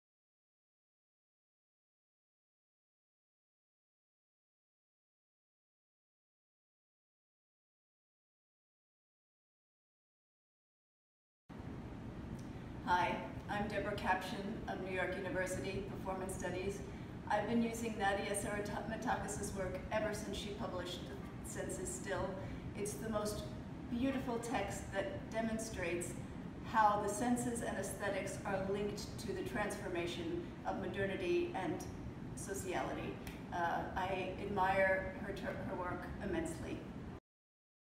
18 Deborah KAPCHAN
WORLD JOURNEYS THROUGH PEOPLE AND TEXTS
                                   The Anthropological Works of
                                            C. Nadia Seremetakis

                           (30 speakers/ video/ photography/ jazz music)

                                                                          Athens, June 10, 2019
                                                                    Garden café of Ilion Melathron